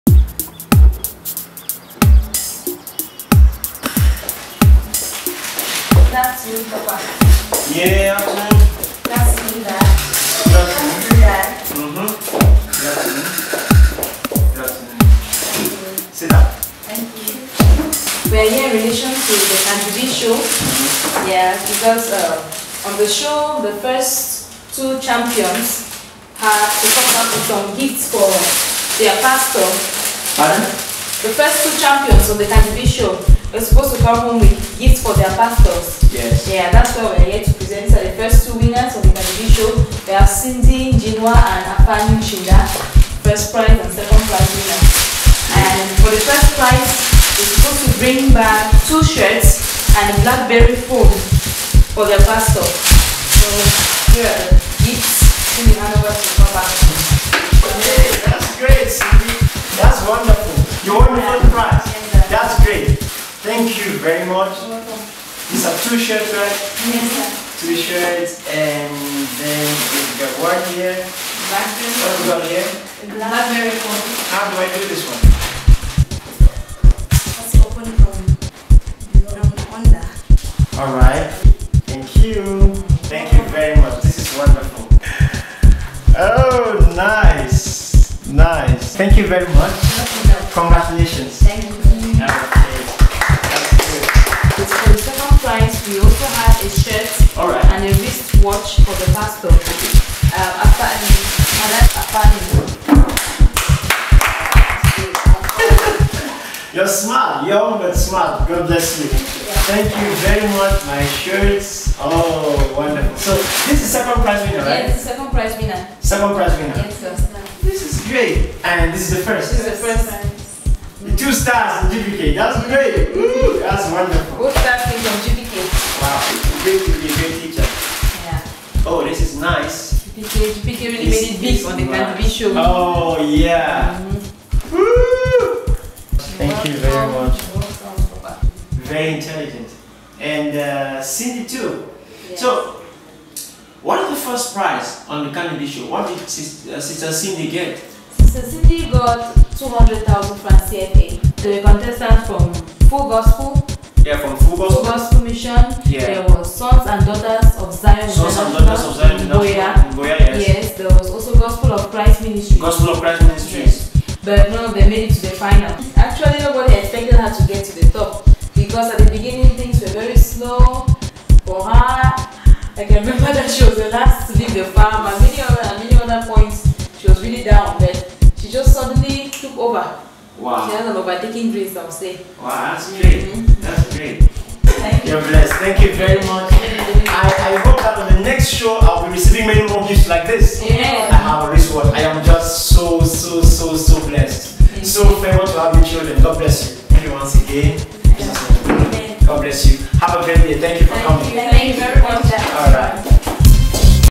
KB Season 1 Gifts!
HONOUR TO WHOM HONOUR IS DUE! Winners Take trophy to their Pastor.